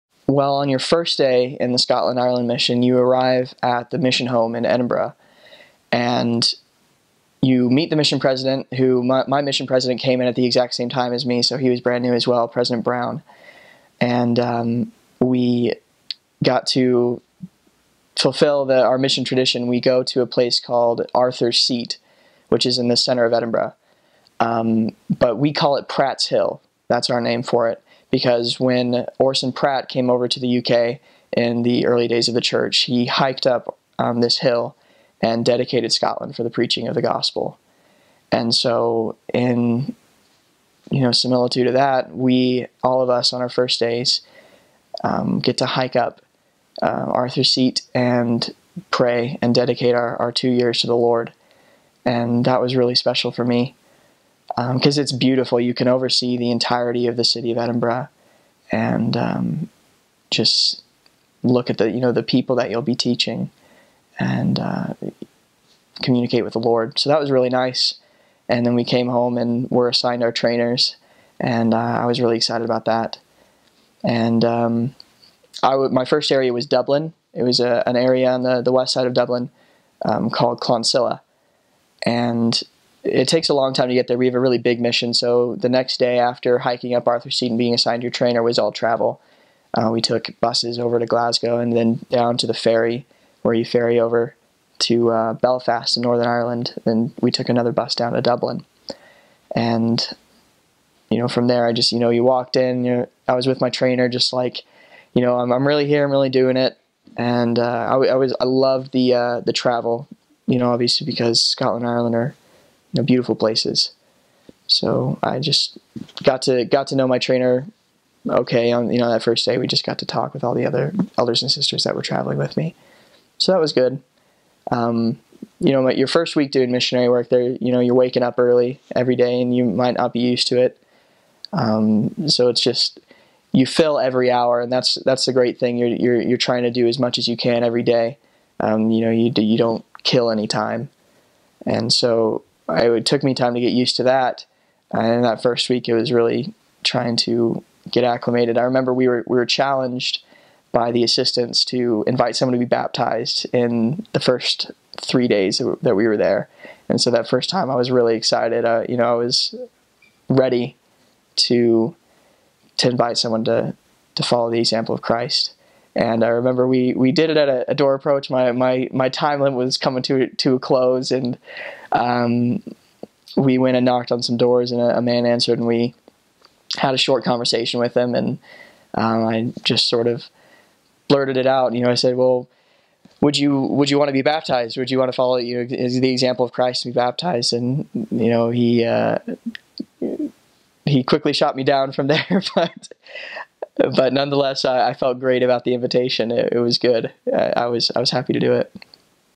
Arriving to the Scotland Ireland Mission
Arriving to the Scotland Ireland Mission (He served a mission there for The Church of Jesus Christ of Latter-day Saints)